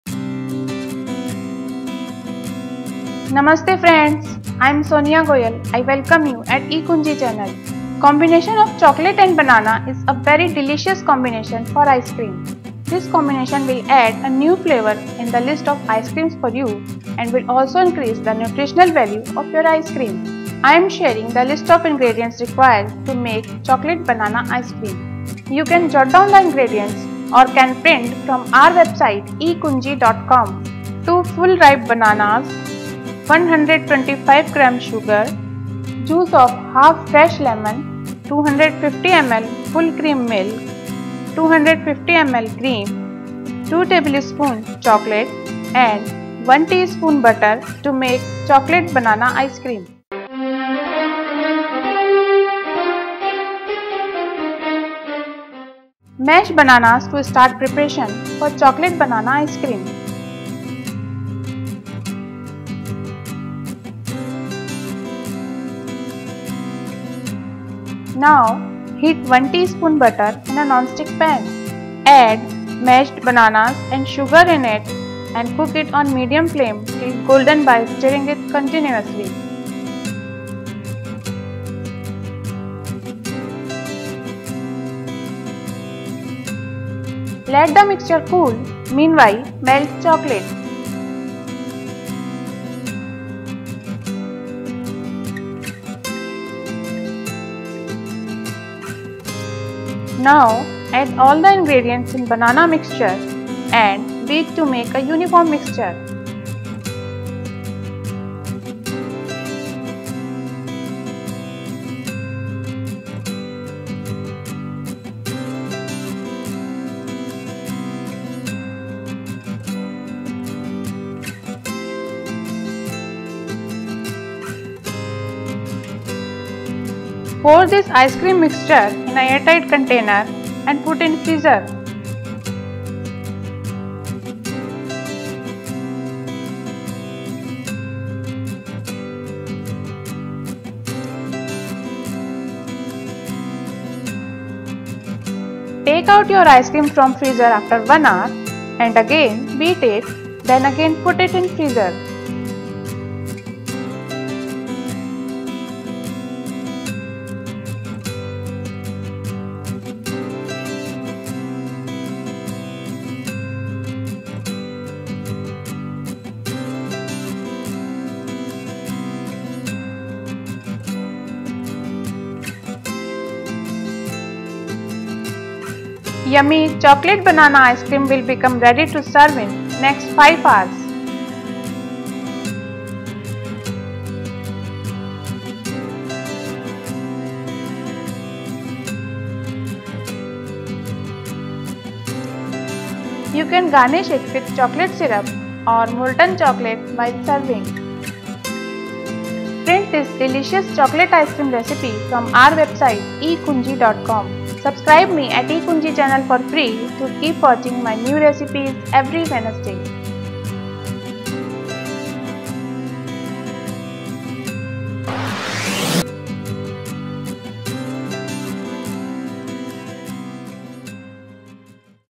Chocolate Banana Ice Cream Recipe By Sonia Goyal @ ekunji.com
Watch chocolate banana ice cream recipe by Sonia Goyal.

Chocolate banana ice cream is the perfact healthy dessert for everyone and certainly satisfy your craving for ice cream. A crumbly, creamy, and fluffy textured ice cream, works as a special treat.

This simple recipe uses bananas as the base, sweetener, and creamy component to homemade ice cream, and you don’t even need an ice cream maker! You would never guess how simple it is to make.

Let's experiment with the detailed method to make this sweet and creamy chocolate banana ice cream in your own kitchen.
Artist : Topher Mohr and Alex Elena
Genre : Rock